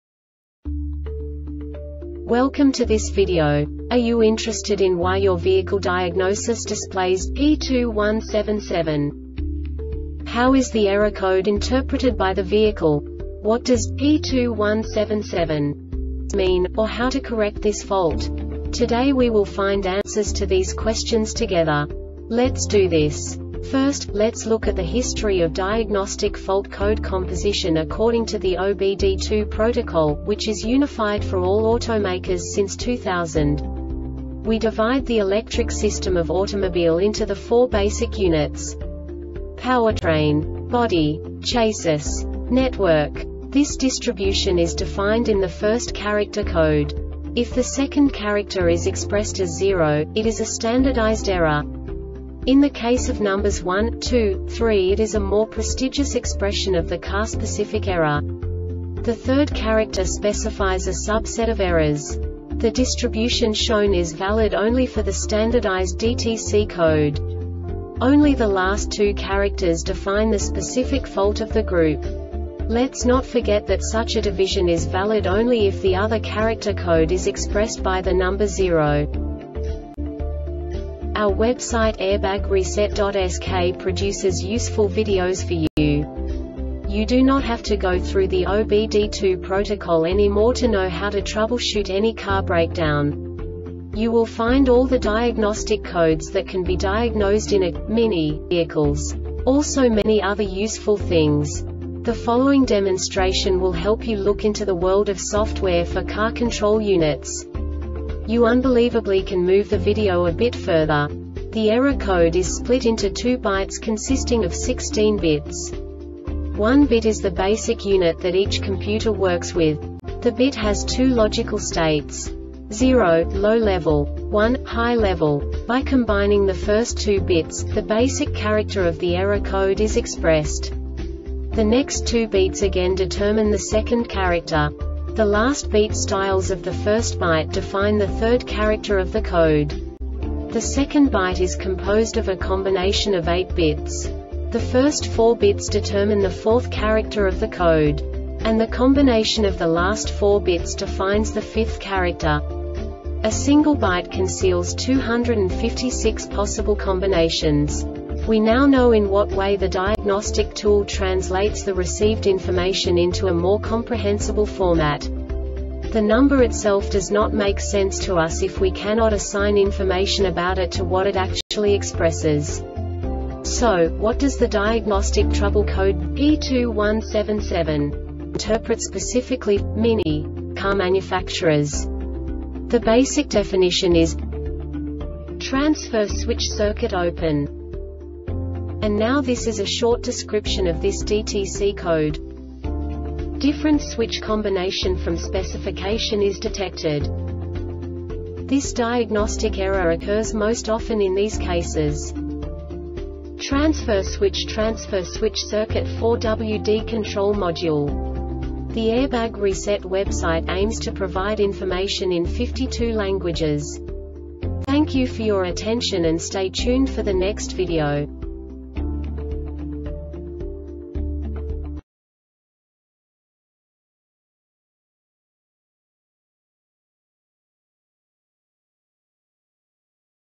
DTC Mini P2177 Short Explanation
Contents:
0:21 Basic DTC analysis according to OBD2 protocol standard.
1:48 Insight into programming
2:58 A diagnostically understandable description of the Diagnostic Trouble Code
3:05 Basic error definition

Make
MINI
Code
P2177
Definition
Transfer Switch Circuit Open
Description
Different switch combination from specification is detected
Cause
Transfer switchTransfer switch circuit4WD control module